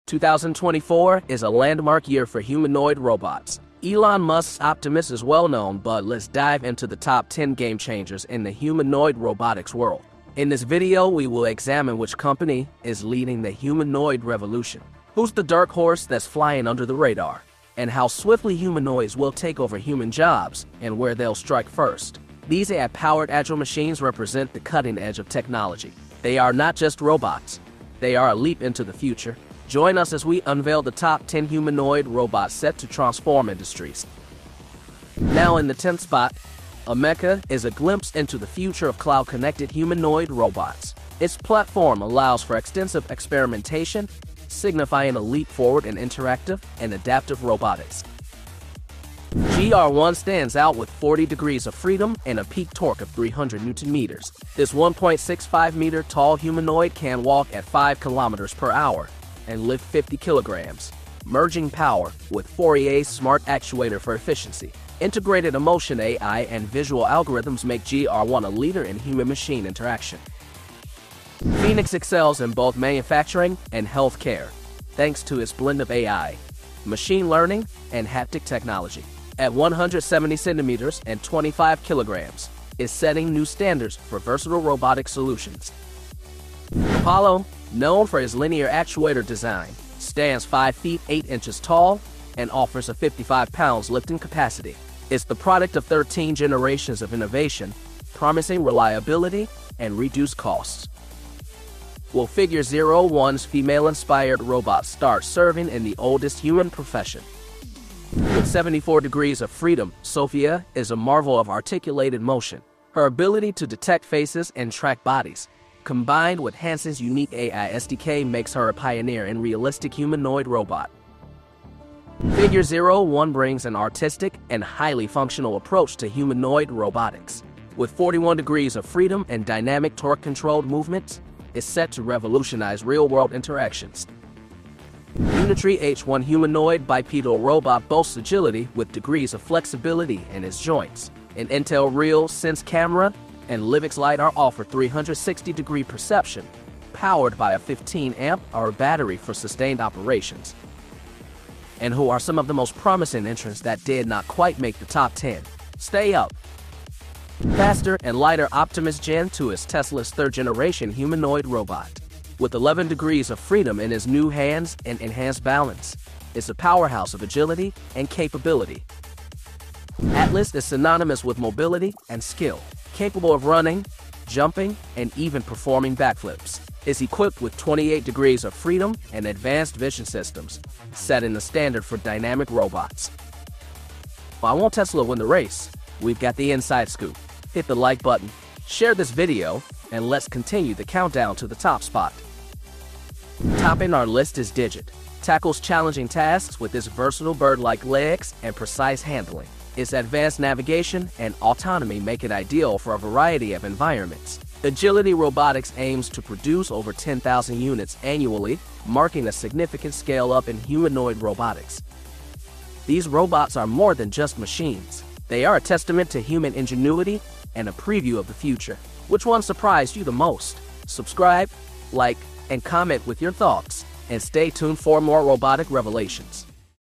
Top 10 Humanoid Robots in 2024 | Disney vs Tesla Optimus Gen 2
The year 2024 is set to be a groundbreaking era for humanoid robots, with a focus on their diverse applications and capabilities. This video unveils the top 10 humanoid robots making waves in the tech world. From Fourier Intelligence's GR-1 with emotion AI to Sanctuary A.I.'s Phoenix revolutionizing labor solutions, the lineup showcases impressive features and functionalities. Plus, additional promising robots that didn't make the top 10.

00:00 Intro

0:11 Who’s Taking Your Job

0:23 Not Just Robots

0:35 Cloud Connected Humanoid

0:49 Emotion AI Integration

1:11 Manufacturing and Healthcare

1:27 Linear Actuator Design

1:48 Articulated Motion

2:02 Artistic and Functional

2:15 Joints Flexibility

2:37 Faster and Lighter

2:50 Mobility and Skill

3:13 Bird Inspired

3:33 More Than Machines

2024 is a landmark year for humanoid robots. Elon Musk's Optimus is well-known but let's dive into the top 10 game-changers in the humanoid robotics world.

In this video we will examine which company is leading the humanoid revolution? Who’s the dark horse that's flying under the radar? And how swiftly humanoids will take over human jobs and where they'll strike first?

These AI powered agile machines represent the cutting edge of technology. They are not just robots, they are a leap into the future. Join us as we unveil the top 10 humanoid robots set to transform industries.

Now in the 10th spot, Ameca is a glimpse into the future of cloud-connected humanoid robots. Its platform allows for extensive experimentation, signifying a leap forward in interactive and adaptive robotics.

GR-1 stands out with 40 degrees of freedom and a peak torque of 300 Nm. This 1.65-meter tall humanoid can walk at 5 kph and lift 50 kg, merging power with Fourier's Smart Actuator for efficiency. Integrated emotion AI and visual algorithms make GR-1 a leader in human-machine interaction.

Phoenix excels in both manufacturing and healthcare, thanks to its blend of A.I., machine learning and haptic technology. At 170 cm and 25 kg, it's setting new standards for versatile robotic solutions.

Apollo, known for its linear actuator design, stands 5 feet 8 inches tall and offers a 55 lbs lifting capacity. It's the product of 13 generations of innovation, promising reliability and reduced costs.

Will Figure 01's female-inspired robots start serving in the oldest human profession?

With 74 degrees of freedom, Sophia is a marvel of articulated motion. Her ability to detect faces and track bodies, combined with Hanson's unique AI SDK, makes her a pioneer in realistic humanoid robots.

Figure 01 brings an artistic and highly functional approach to humanoid robotics. With 41 degrees of freedom and dynamic torque-controlled movements, it's set to revolutionize real-world interactions.

Unitree H1 humanoid bipedal robot boasts agility with degrees of flexibility in its joints. An Intel RealSense camera and Livox LiDAR offer 360-degree perception, powered by a 15 amp-hour battery for sustained operations.

And who are some of the most promising entrants that did not quite make the top 10? Stay up!

Faster and lighter, Optimus Gen 2 is Tesla's third-generation humanoid robot. With 11 degrees of freedom in its new hands and enhanced balance, it's a powerhouse of agility and capability.

Atlas is synonymous with mobility and skill. Capable of running, jumping, and even performing backflips, it's equipped with 28 degrees of freedom and advanced vision systems, setting the standard for dynamic robots.

Why won't Tesla win the race? We've got the inside scoop.

Topping our list is Digit, tackles challenging tasks with its versatile, bird-like legs and precise handling. Its advanced navigation and autonomy make it ideal for a variety of environments. Agility Robotics aims to produce over 10,000 units annually, marking a significant scale-up in humanoid robotics.

These robots are more than just machines, they are a testament to human ingenuity and a preview of the future. Which one surprised you the most?

Using robotics and AI to boost business growth? This video is for you! 🔔